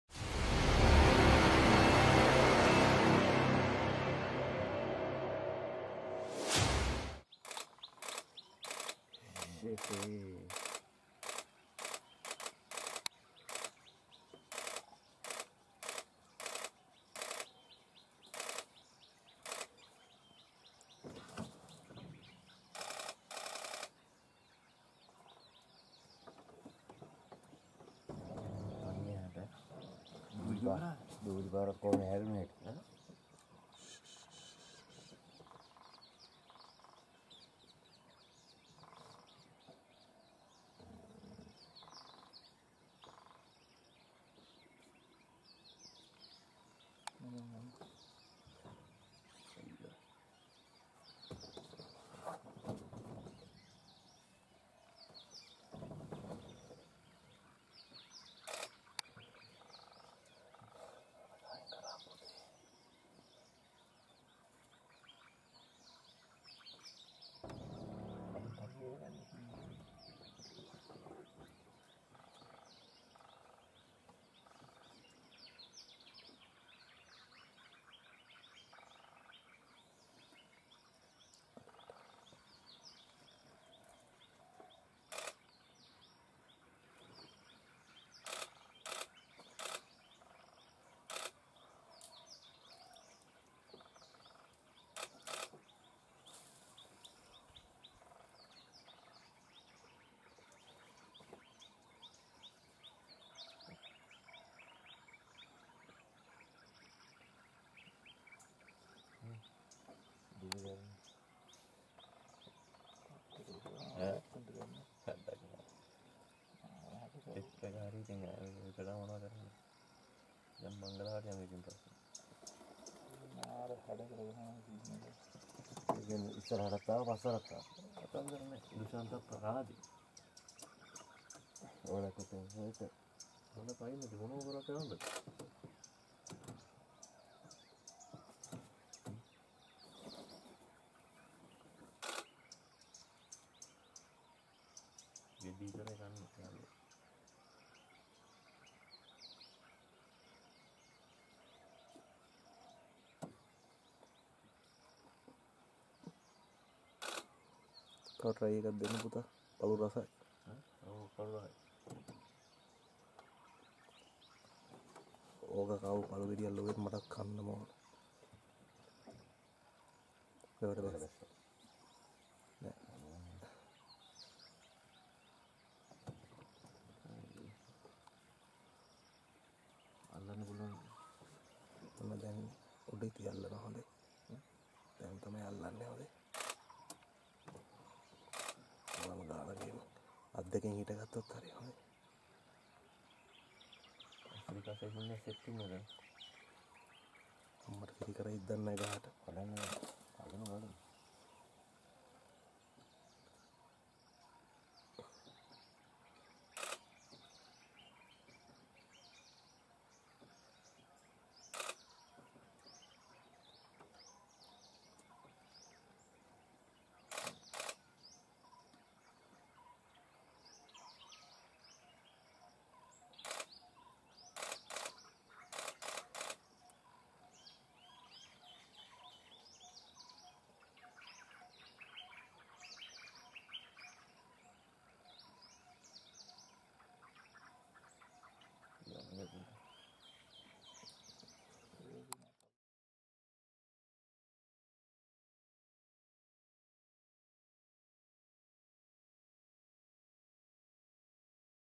Rare tusker is enjoying seasonal fruit called Palu
Rare tusker is enjoying seasonal fruit called Palu (Manilkara hexandra)